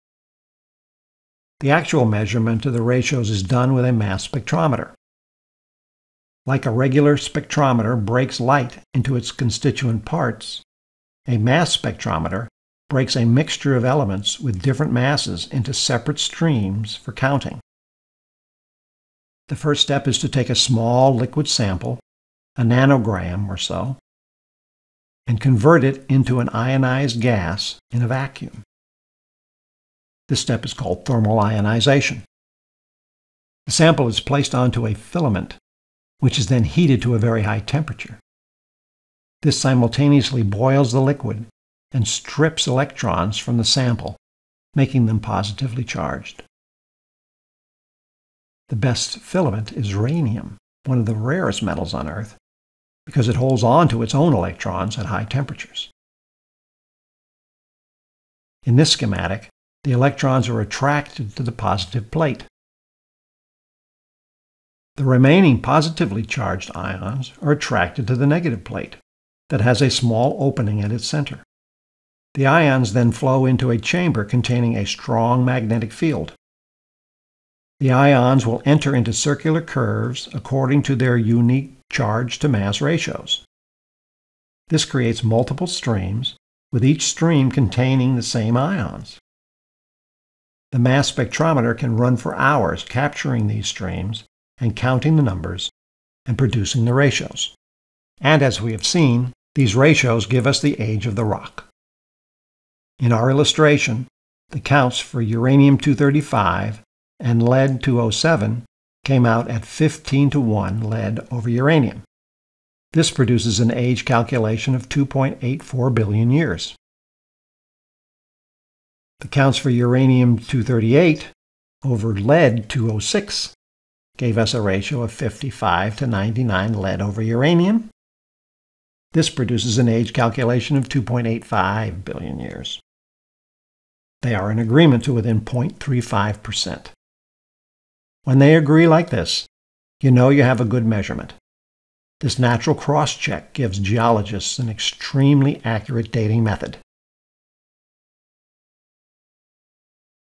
Classroom Aid - Mass Spectrometer
In this chapter we cover the Giant Impact Hypothesis as the starting point for the age of the modern Earth-Moon system. We’ll see a Fulldome illustration on what it might have looked like. We then cover Oxygen Fractionation and use water evaporation to show how it works. We build the Terrestrial Fractionation line and show that all oxygen on Earth falls on this line. We then show that meteors like Allende do not fit on the line, but Moon rocks do. We then begin our deeper dive into how we date rocks using Radiometric Dating with uranium and lead. We review the discovery of radiation with its alpha, beta and gamma rays. We cover half-life and the exponential law of radioactive decay including the uranium to lead decay chains. We also cover the Chemical Abrasion - Isotope Dilution - Thermal Ionization Mass Spectrometry (CA-ID-TIMS) method for dating zircon crystals in rocks found here on Earth. This includes a look at how the Memorial University of Newfoundland does it. We end with the age of the oldest Earth and Moon rocks.